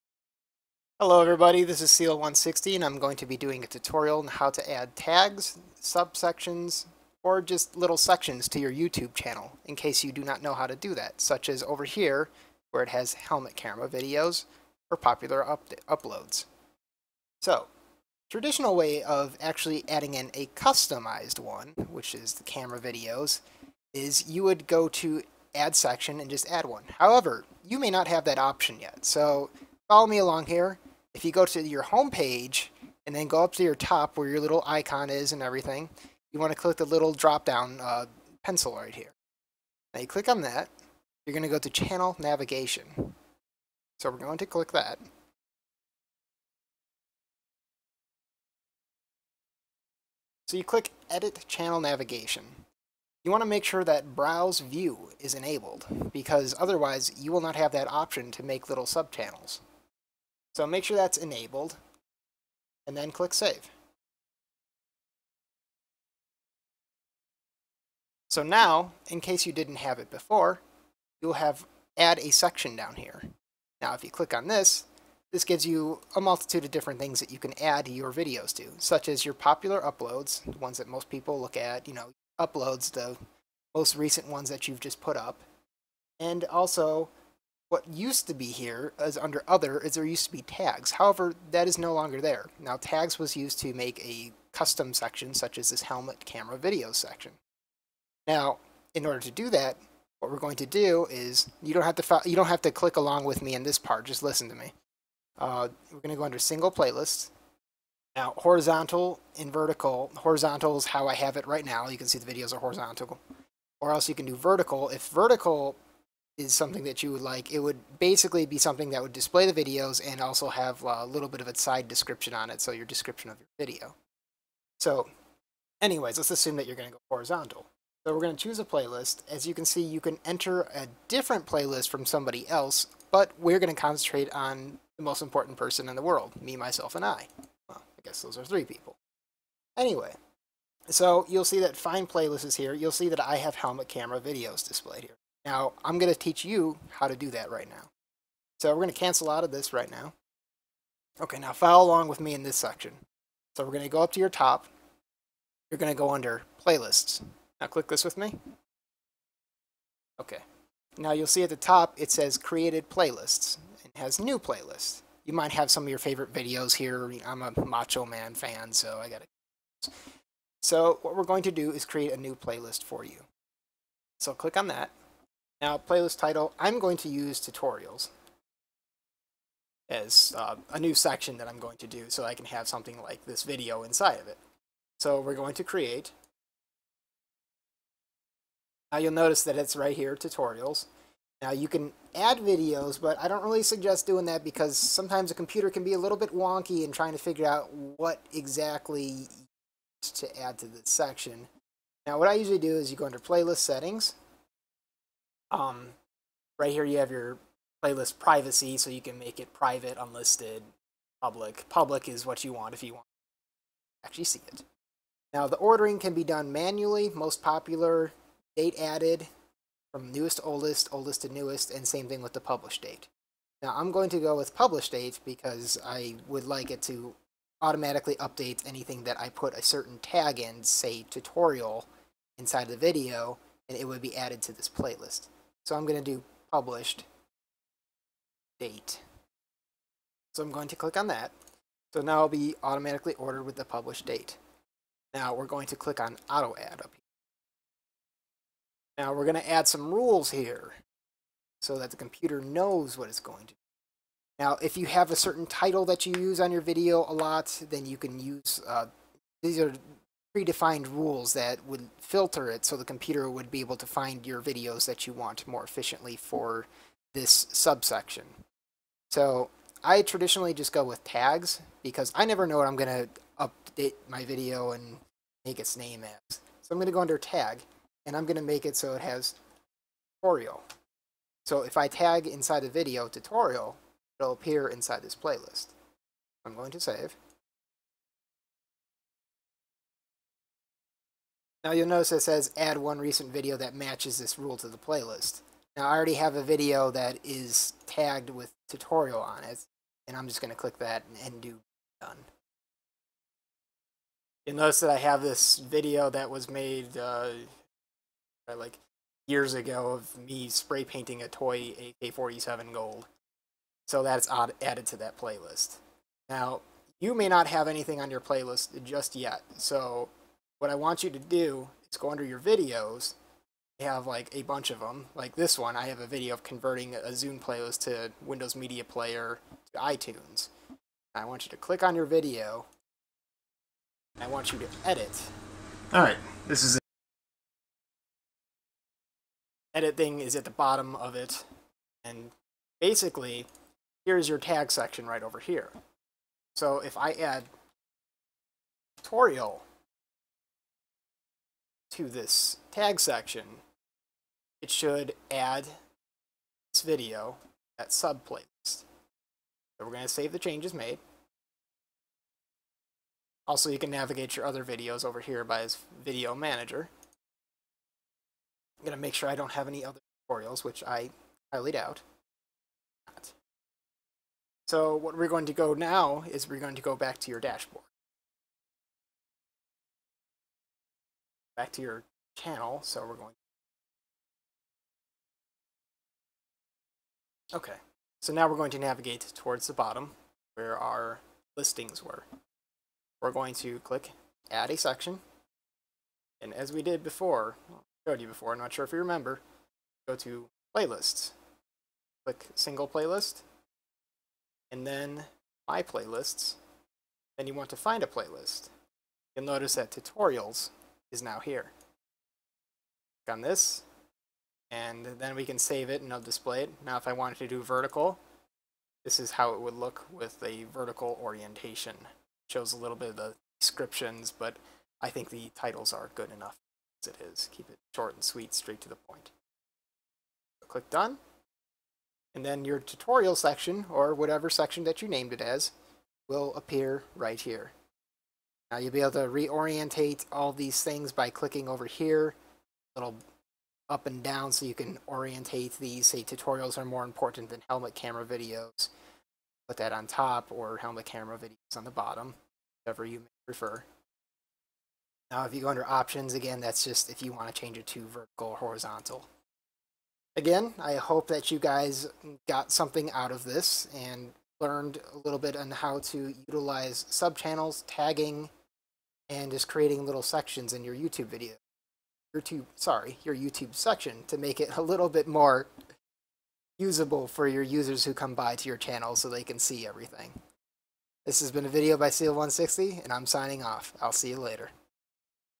How to Create a Youtube Subchannel, Subsection, Tag, Playlist
This video will walk you through how you can make a customized subsection of Videos on your Youtube account which includes your Youtube videos that are categorized.

-Seal160